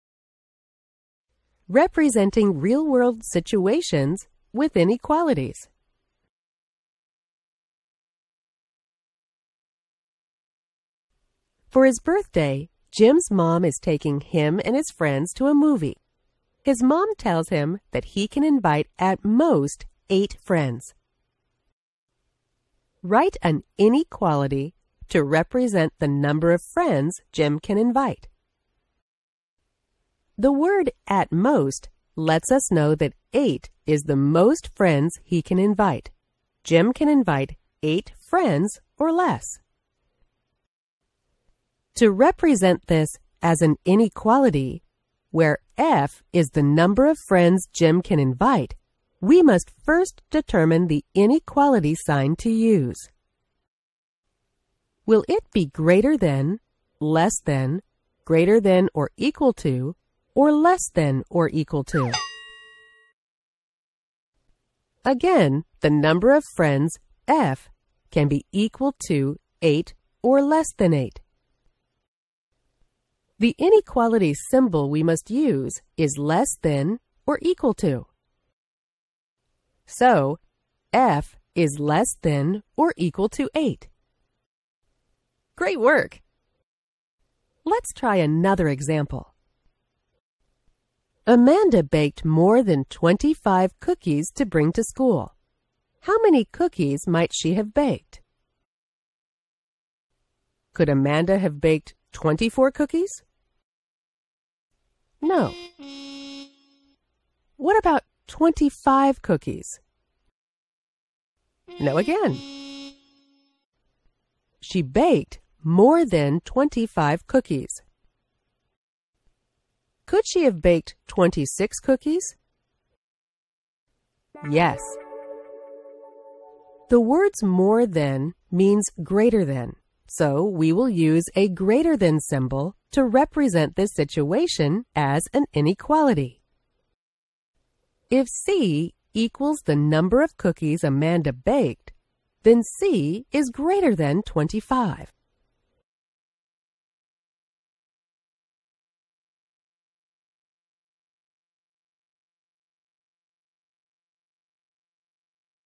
Representing Real-World Situations with Inequalities (Part 1)
Represent real-world situations with inequalities.

Now free and open to everyone.